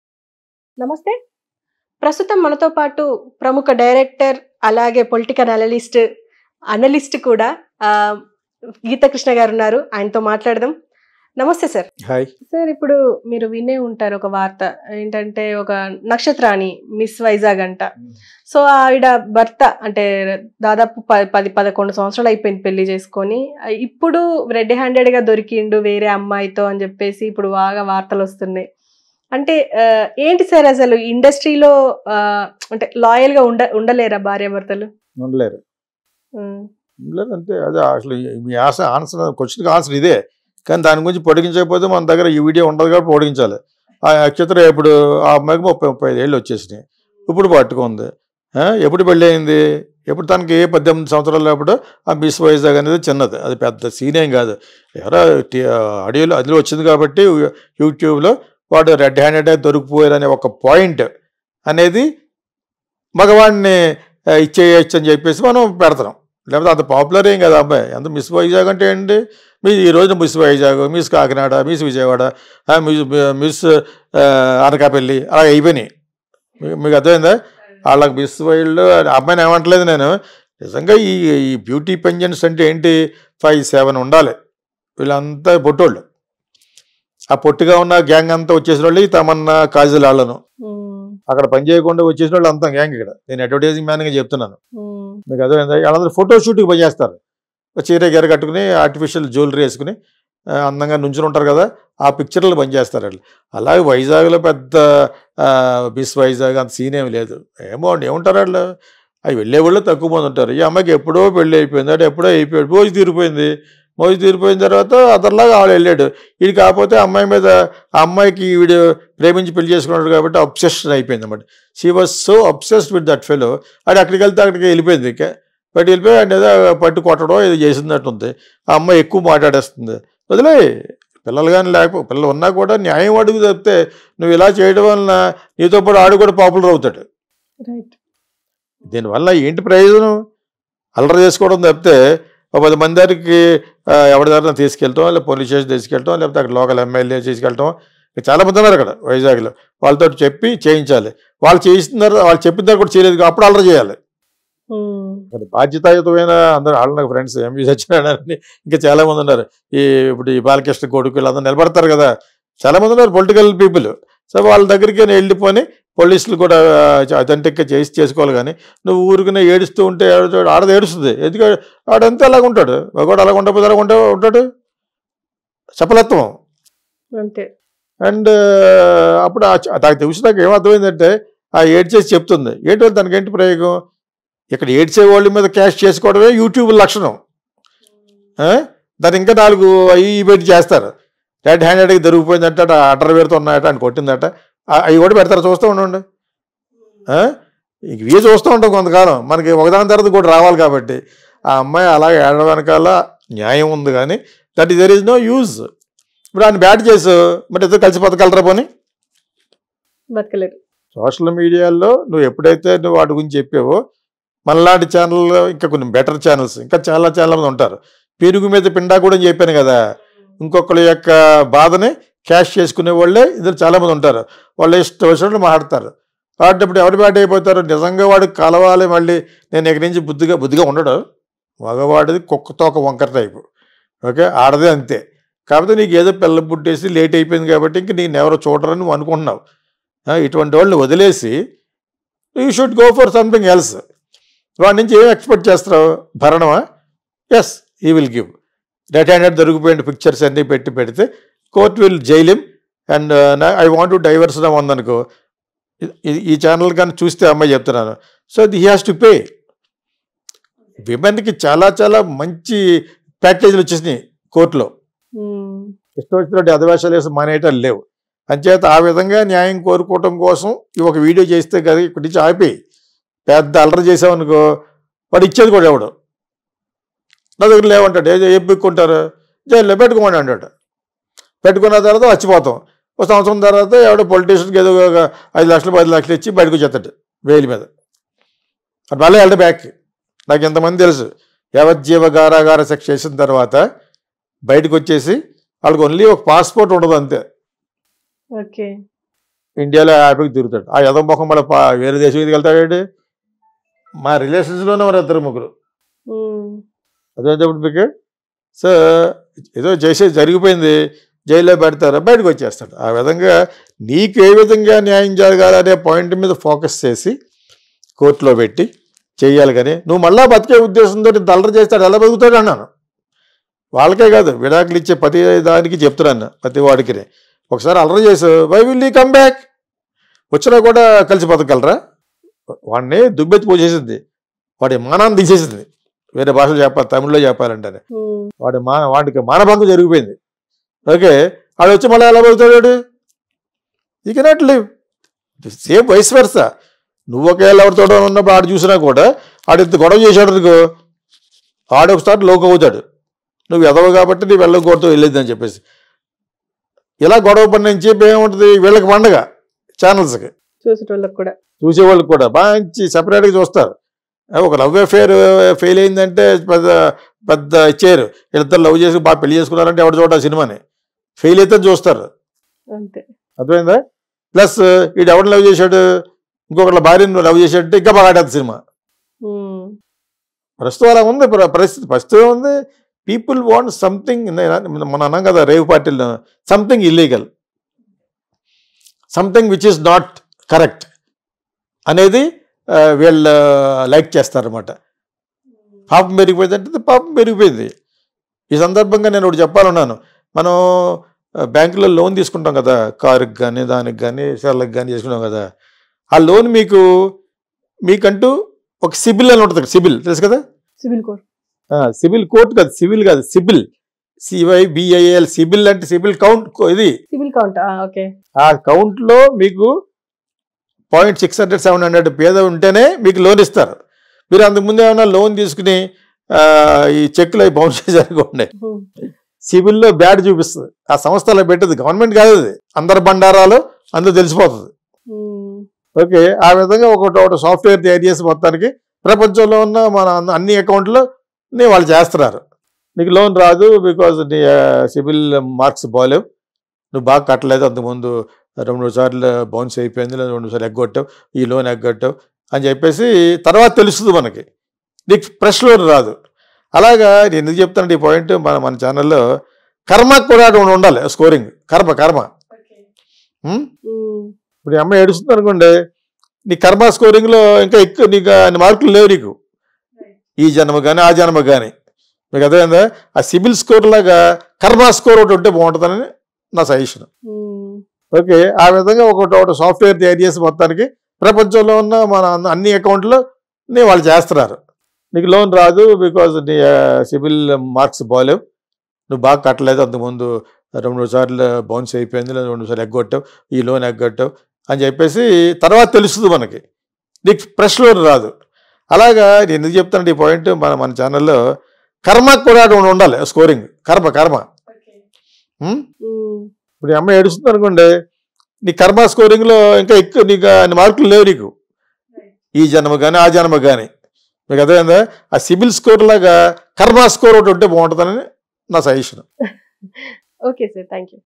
Director Geetha Krishna About Miss Vizag Nakshatra Husband With Another Woman In The Room
Director Geetha Krishna About Miss Vizag Nakshatra Husband With Another Woman In The Room | RED TV TELUGU